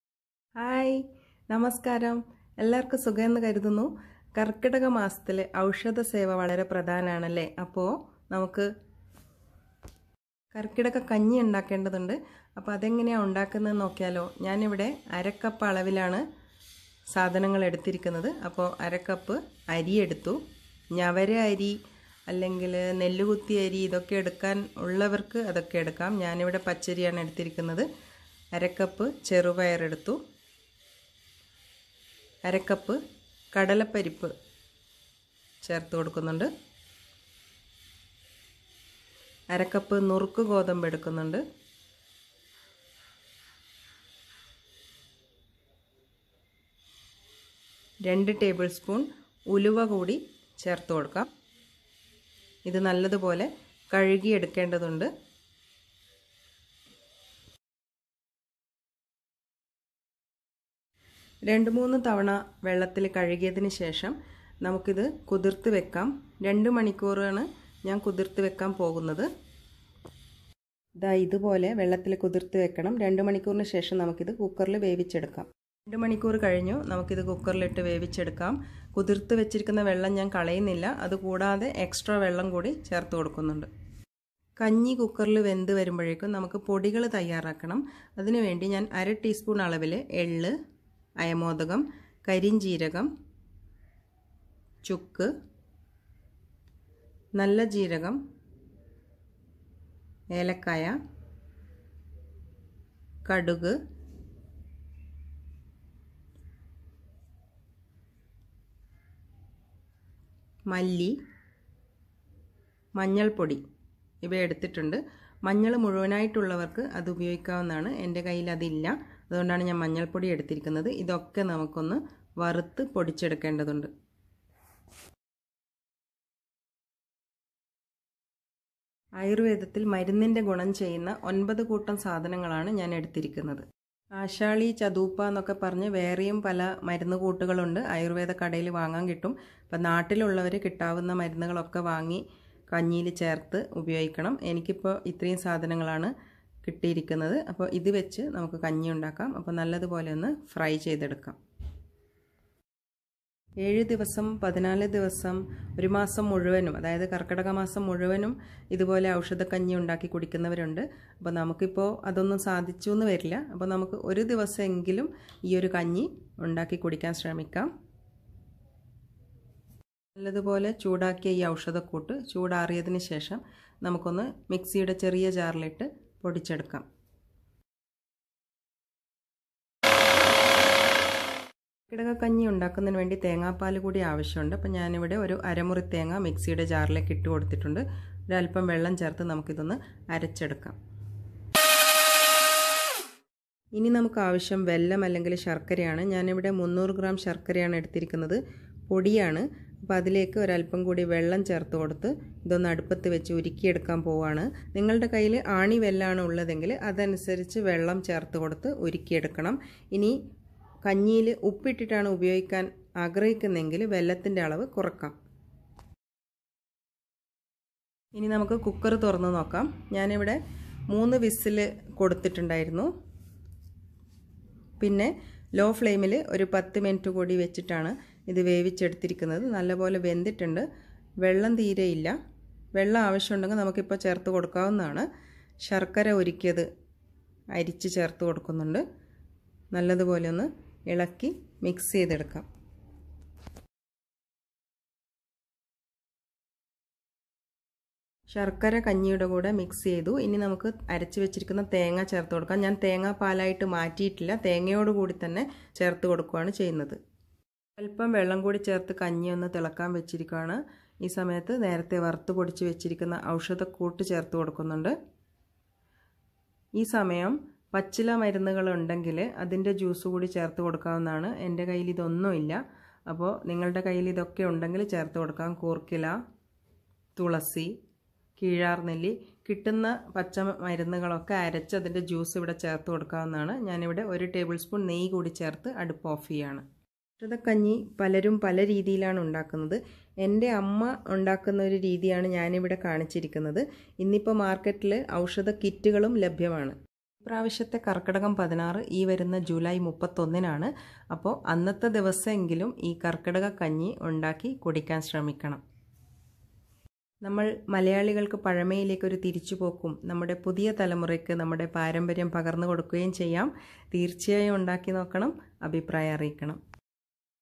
കർക്കിടകക്കഞ്ഞി /ഔഷധക്കഞ്ഞി /ayurveda porridge made of healthy ingredients
Ingredients
Rice 1/2cup
Broken wheat /daliya 1/2cup
Bengal gram dal 1/2cup
Moong beans/Green gram 1/2 cup
Fenugreek seeds 2 table spoon

Wash well and soak for 2hrs

After 2hours, put all the soaked items in a cooker, add enough water and cook well. I put in full flame till 3whistles and then in low flame for 10minutes.

Take 1/2teaspoon each of

Carom seeds /ajwain
Black cummin /kalounji
Dry ginger /Soont
Cummin seeds /jeera
Cardamom /elaichi
Mustard seeds /rai
Corriander seeds /dhaniya
Turmeric powder /haldi

Put all together in a pan on flame and roast well. Grind and make powder and keep aside.

Take out milk of coconut using a grinder

Take jaggery about 300gms,  melt and keep aside. If you prefer putting salt then lessen the amount of jaggery.

Now, open the cooker and add the melted jaggery. Mix well. Add the coconut milk. I used coconut and milk as well. Add water as per your need to loosen the porridge or kanji. Bring it to a boil.

Add the powdered medicine. At this time you can add the juice of tulsi /basil, pudina and other medicinal greens.

I don't have anything in my hand so i put only dry medicines

Add 1 -2tablespoon of ghee and put off the flame. Serve hot.

You can continue use of kanji for 7days 14days or for whole karkidaka/ashada /saavan /shraavan  month.

Don't forget to make and have the karkidaka kanji on 31st July.